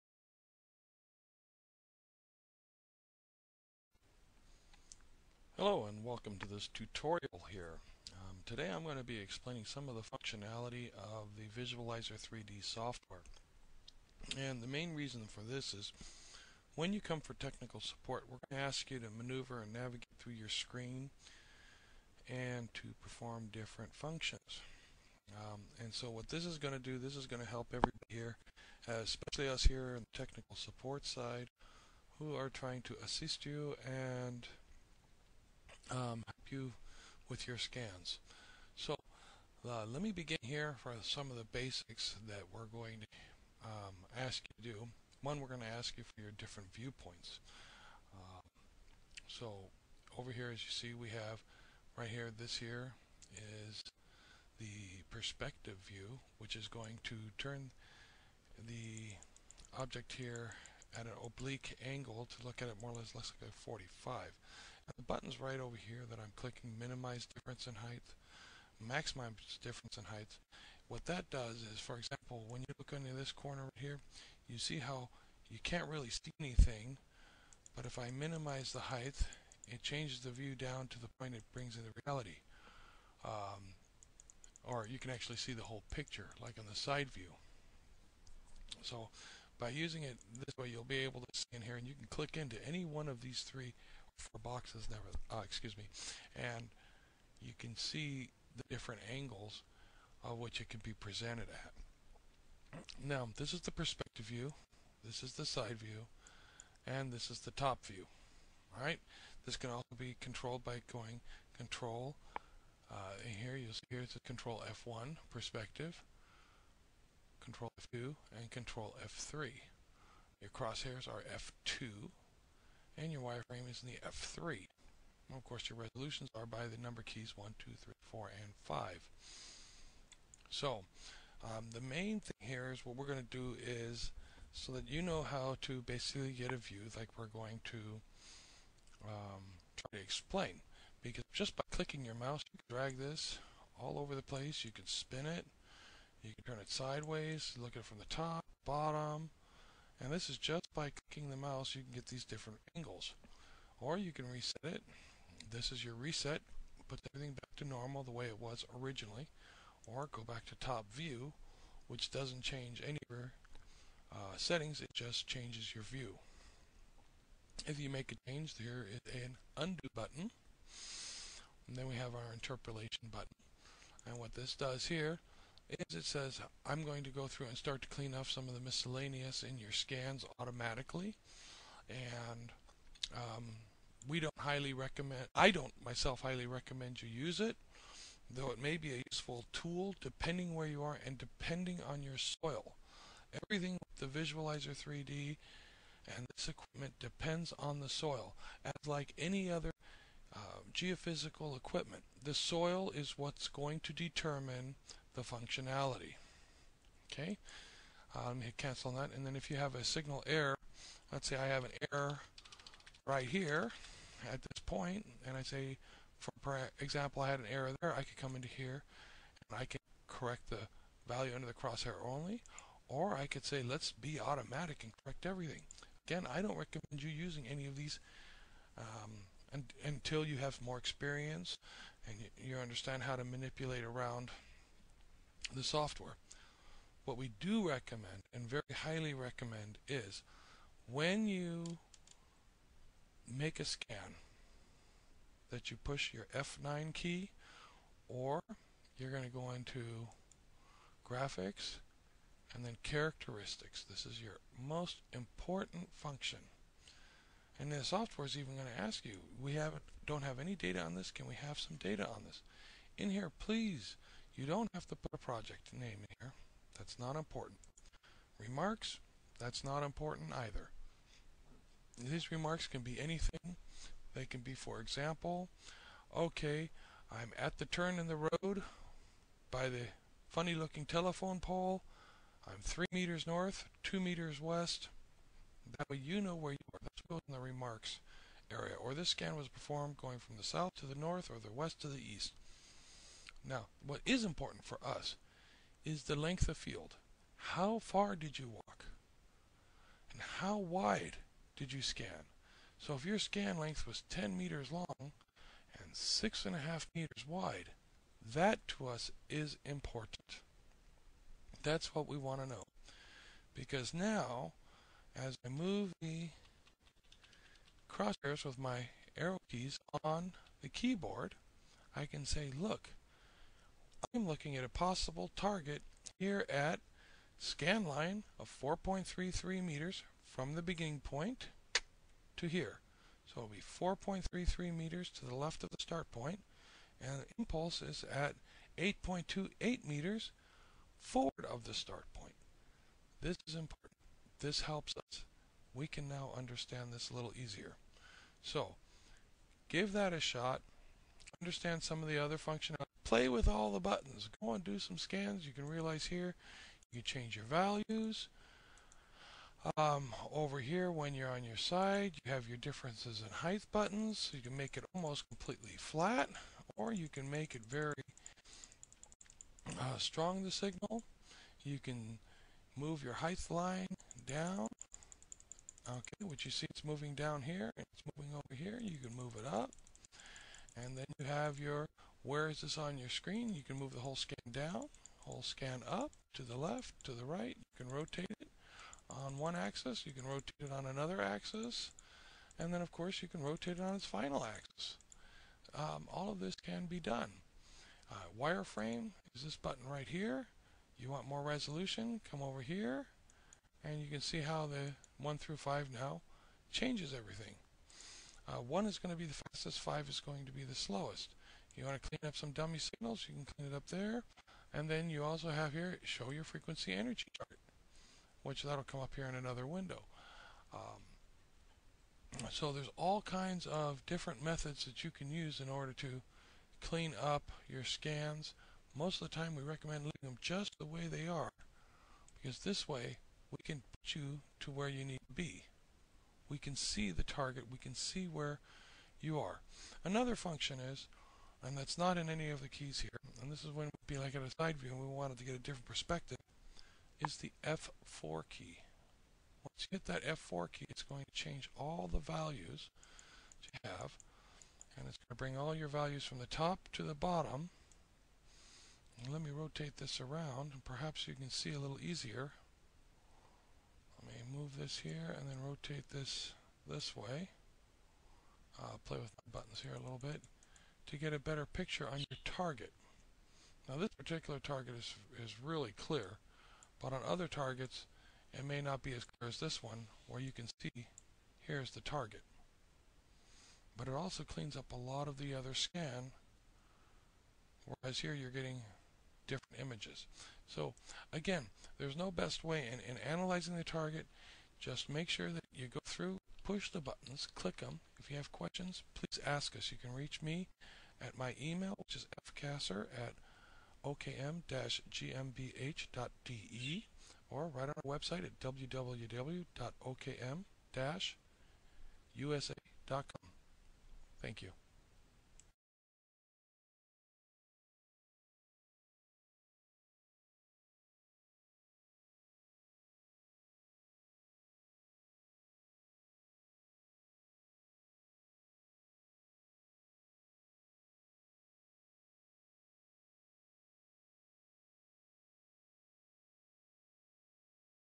Visualizer 3D Software Navigation
Understand the functionality of the Visualizer 3D Software for the OKM line of Metal Detectors.